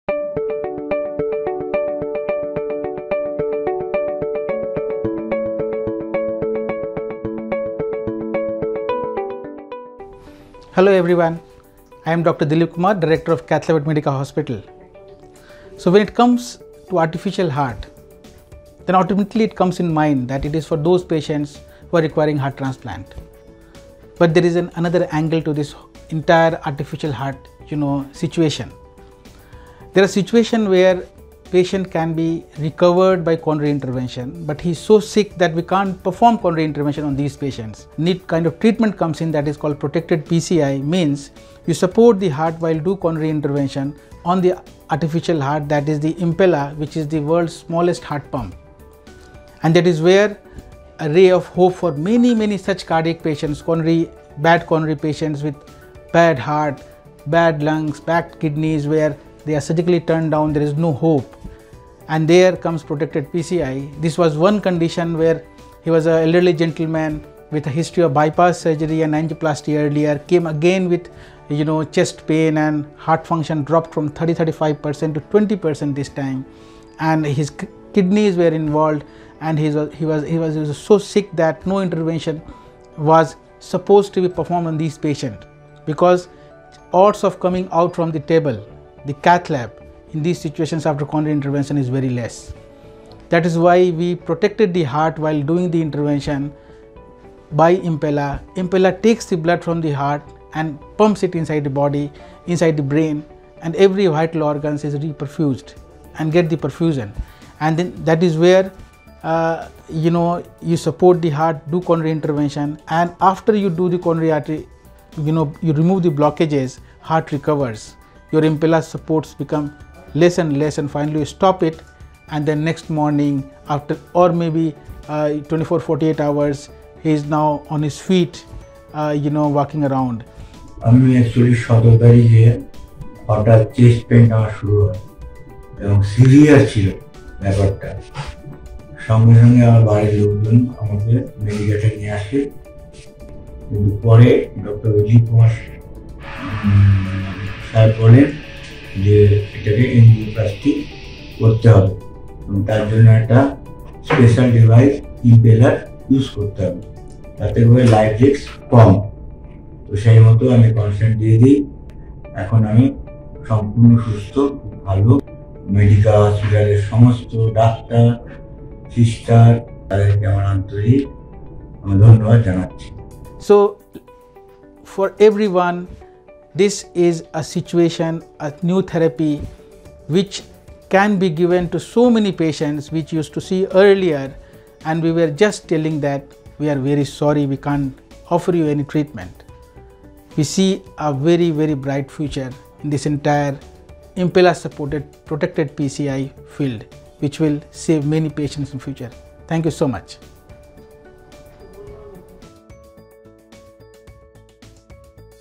India’s 1st Chronic Total Occlusion (CTO) Angioplasty on Impella Support by Dr. Dilip Kumar | Medica
Have you ever heard about the smallest Heart pump that takes blood from the heart and pumps it inside the body?
It is known as Impella, which keeps the heart going while the doctors perform Protected PCI.
Let’s hear it from our Director- Cath Lab, Device and Structural Heart specialist, Dr. Dilip Kumar, himself, who conducted India’s 1st Chronic Total Occlusion (CTO) Angioplasty on Impella Support.